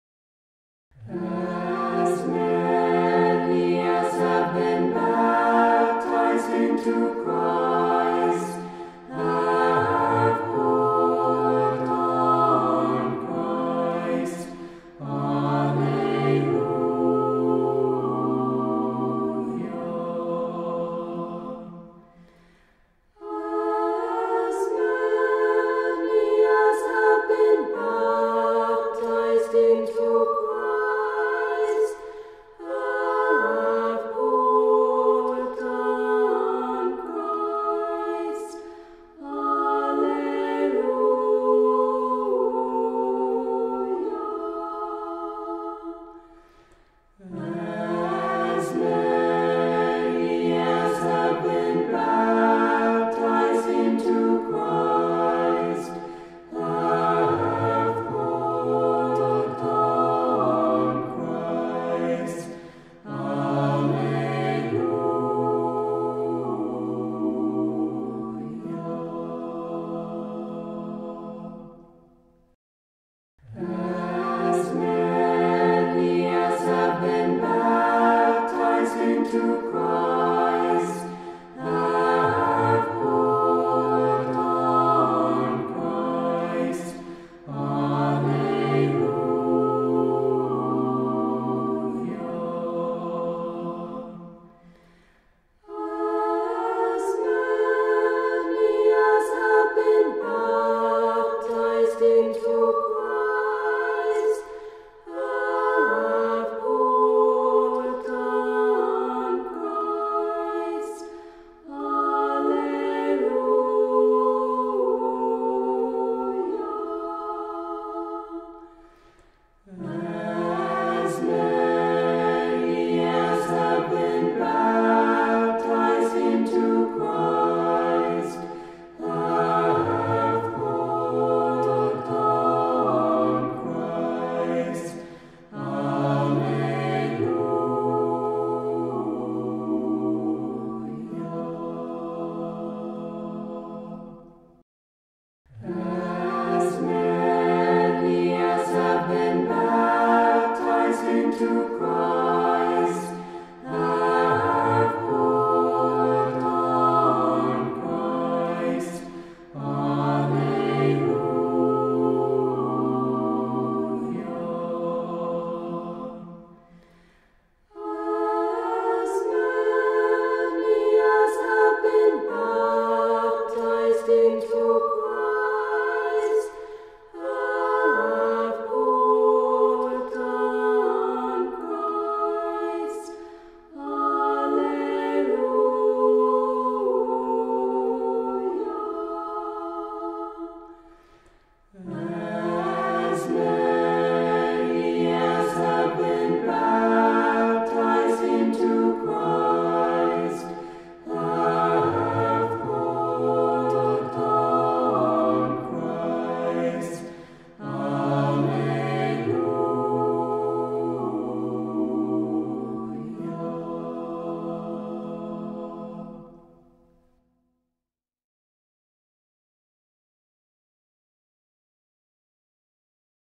Christening.avi
The Christening and Baptism of Elizabeth Jackson on June 27th. May God Grant Her Many Years!